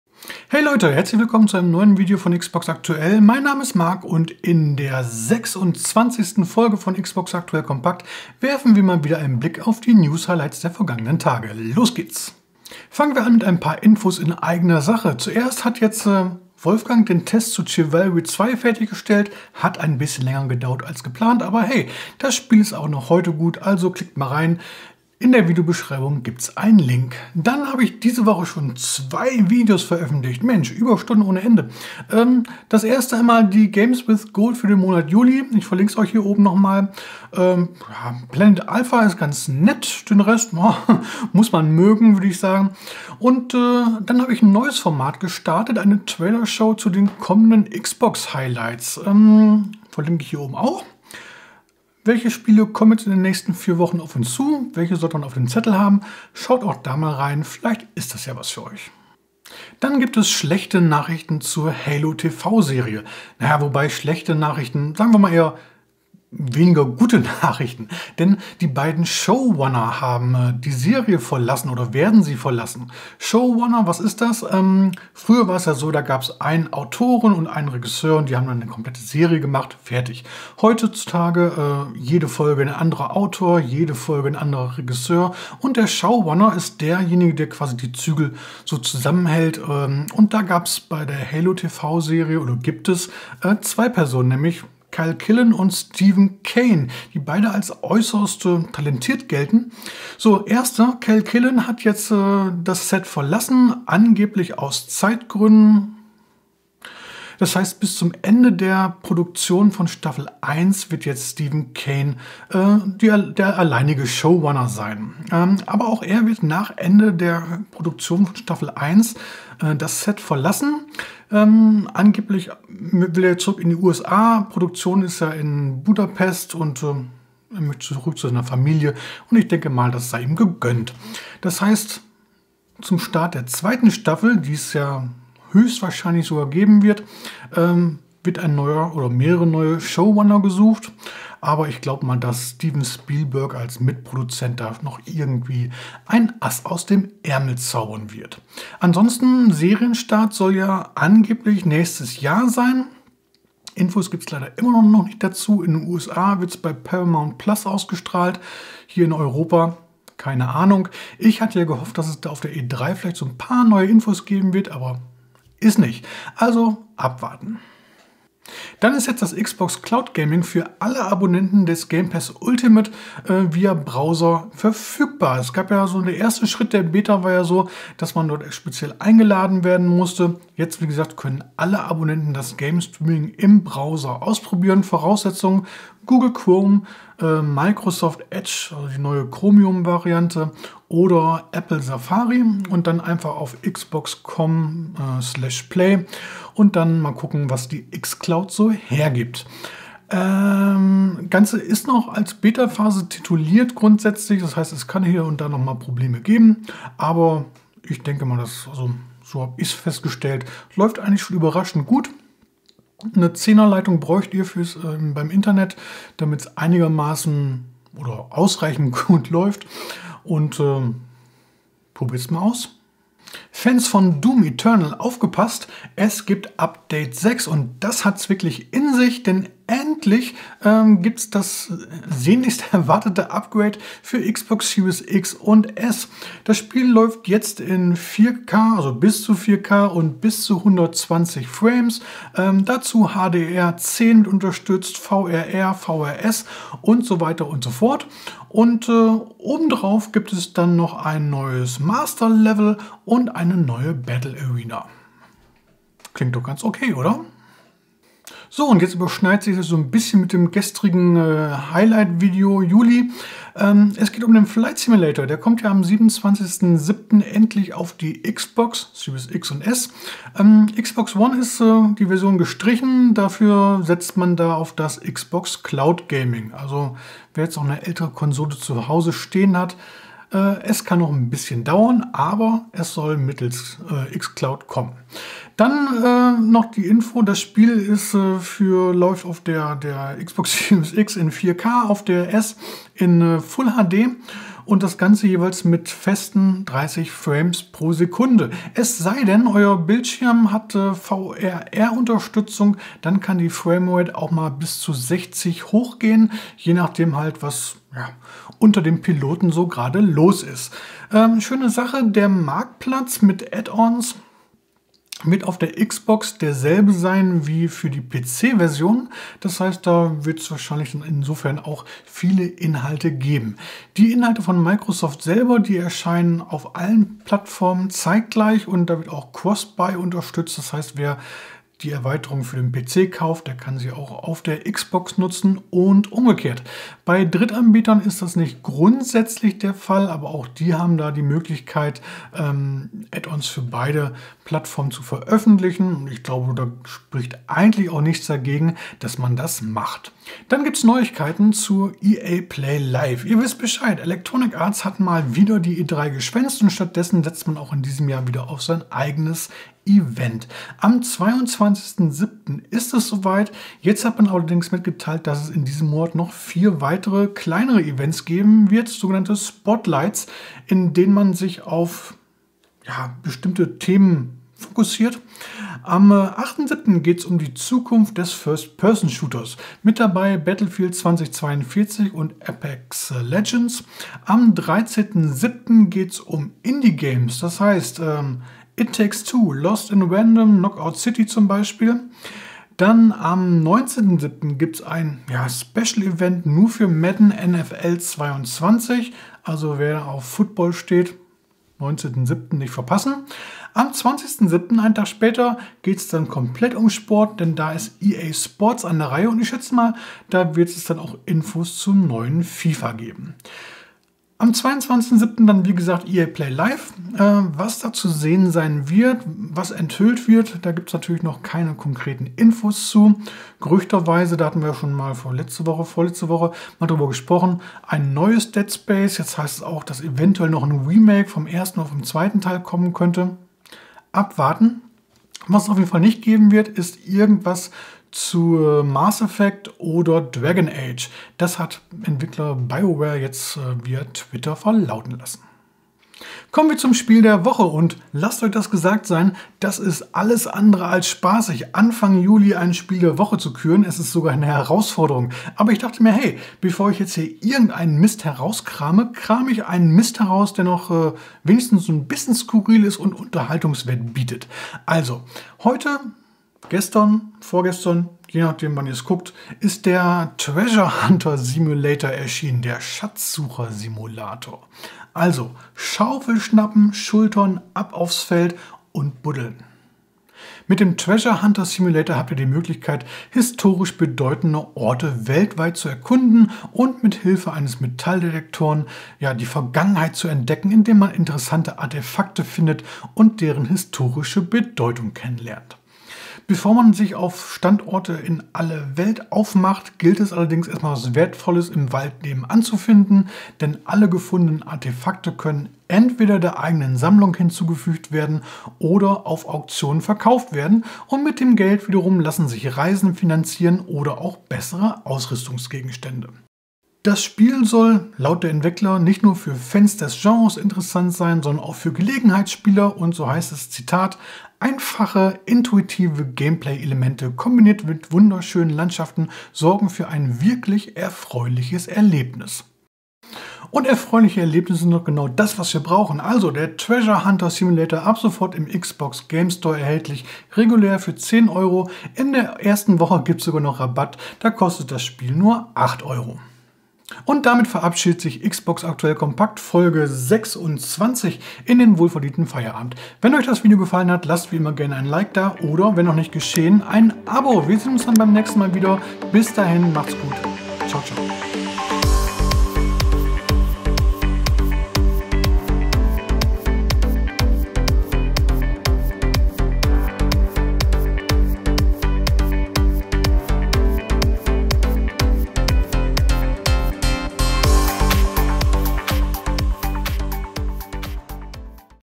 XA Kompakt Folge 26: Die Xbox-News der Woche
In unserem Format "Xbox Aktuell Kompakt" werfen wir einen Blick zurück auf die vergangene Woche und fassen für euch die News-Highlights rund um unsere Lieblingskonsole zusammen.

Diesmal u. a. mit dabei: XboxCloudGaming DoomEternal EAPlayLive und mehr rund um die Xbox​​​​​​​​​​​​​​​

Inhalte dieser Folge im Detail:

00:00 Hey Leute!
00:12 Chivalry 2
00:32 Games With Gold Juli
00:45 Spiele-Highlights Juli
00:59 Halo TV-Serie
02:48 Xbox Cloud Gaming
03:43 Doom Eternal
04:17 Microsoft Flight Simulator
05:50 EA Play Live
07:25 Treasure Hunter Simulator
09:21 Ciao ciao!